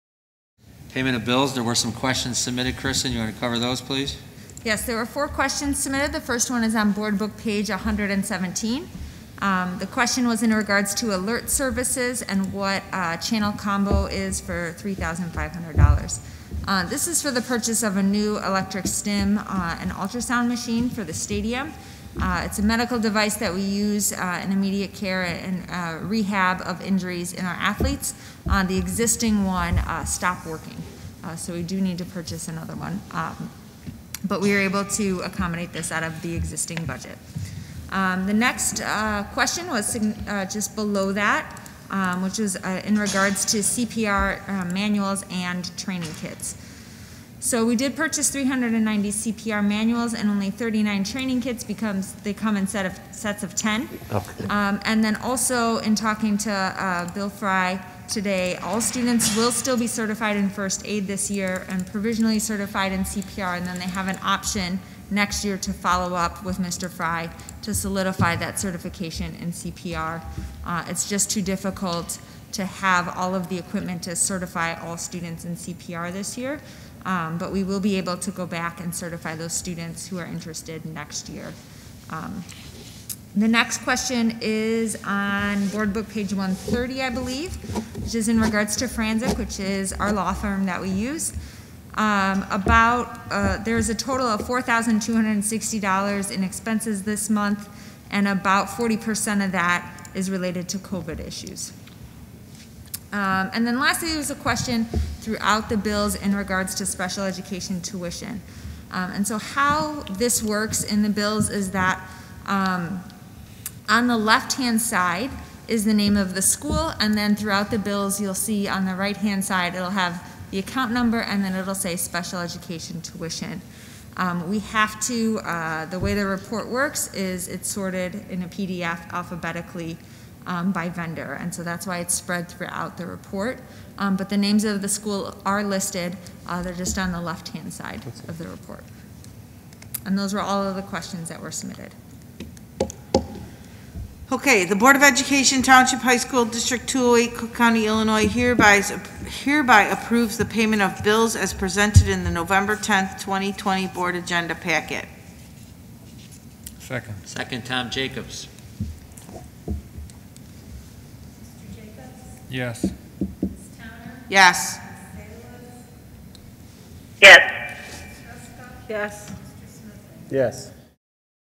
NB Payment of Bills 11-10-20
Part 16 of the November 2020 regular business meeting of the Township High School District 208 Board of Education of Cook County in Riverside, Illinois.

This video was produced by RBTV — Riverside Brookfield High School.